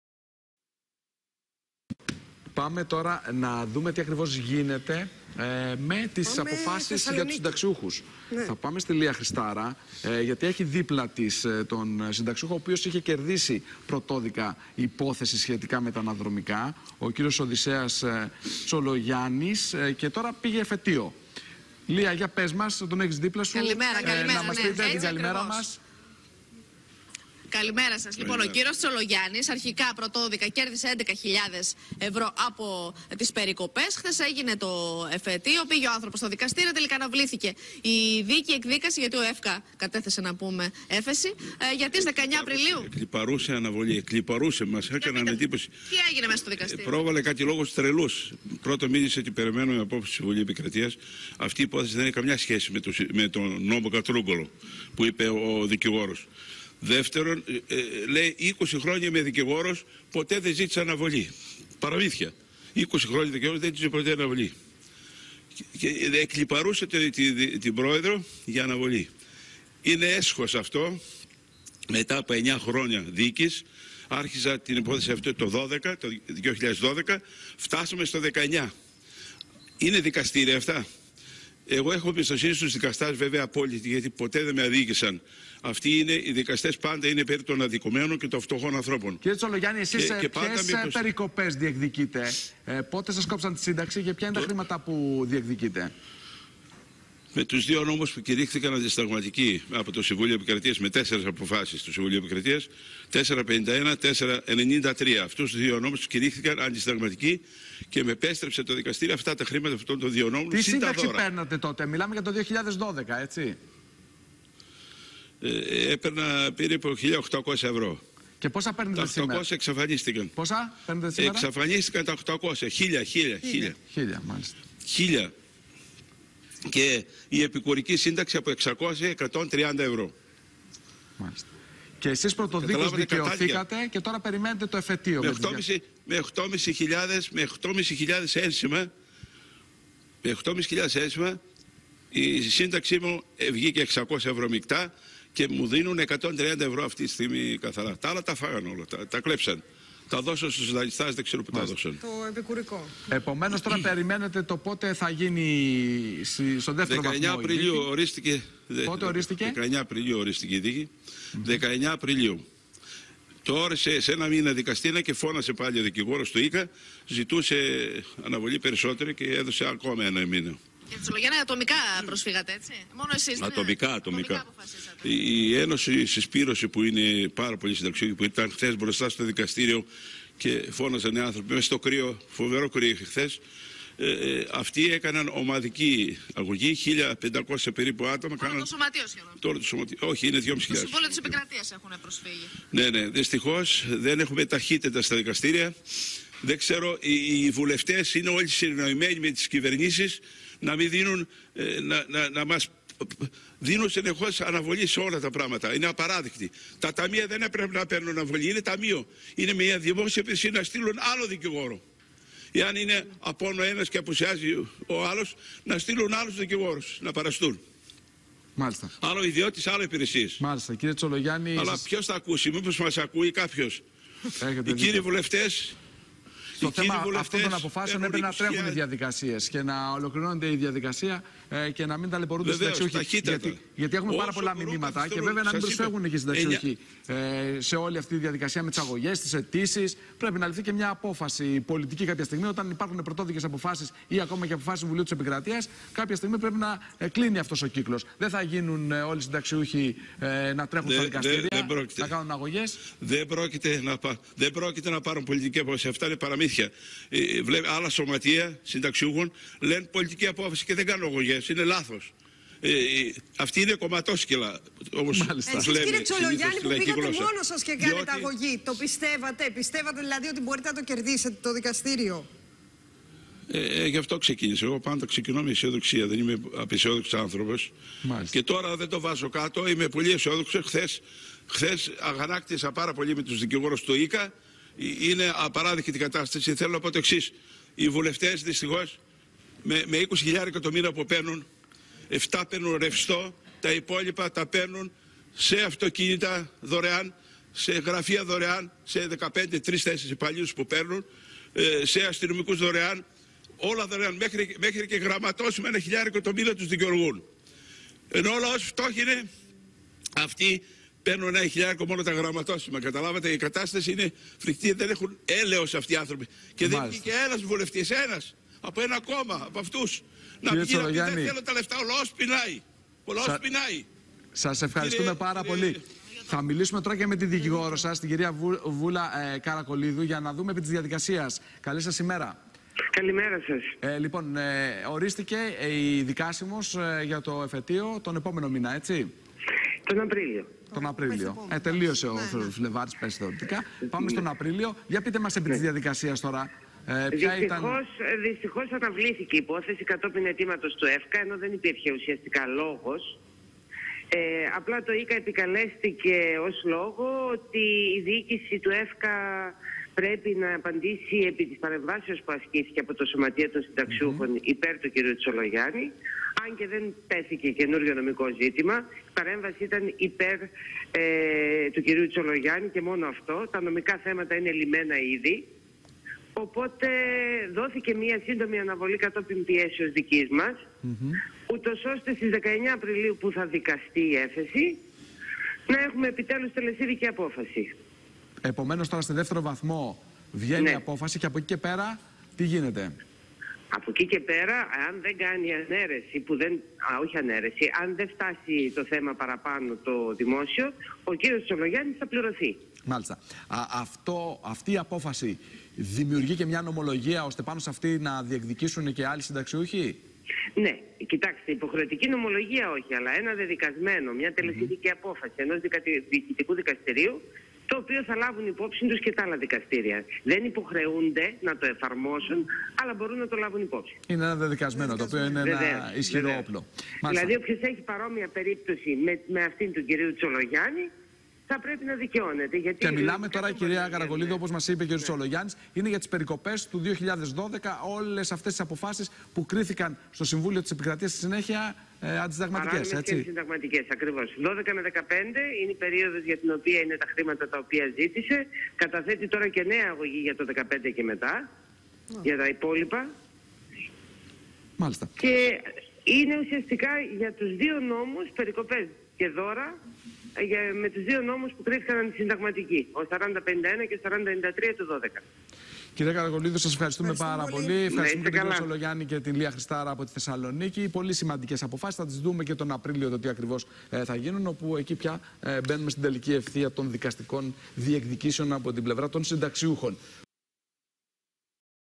Οδυσσέας Τσολογιάννης :Ο ΠΡΩΤΟΣ ΣΥΝΤΑΞΙΟΎΧΟΣ ΠΟΥ ΔΙΚΑΙΩΘΗΚΕ-Αναβολή για τις 19 Απριλίου..
Οδυσσέας Τσολογιάννης:Ο ΠΡΩΤΟΣ ΣΥΝΤΑΞΙΟΎΧΟΣ ΠΟΥ ΔΙΚΑΙΩΘΗΚΕ - Αναβολή για τις 19 Απριλίου πήρε η εκδίκαση έφεσης για τα αναδρομικά συνταξιούχου  ΕΡΤ WebTV